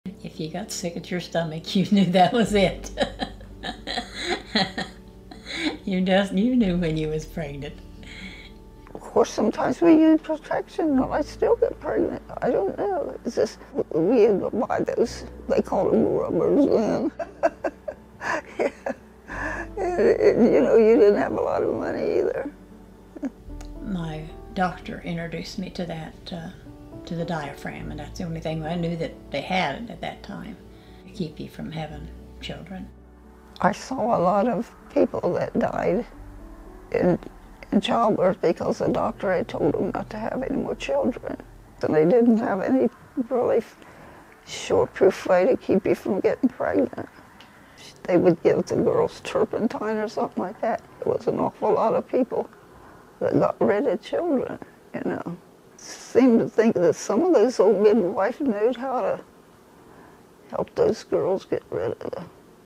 Childbirth Risks & Early Contraception Methods
Discover the dangers women faced during childbirth and early methods of contraception. From limited options to risky solutions, explore the past challenges of family planning. Watch now to learn more!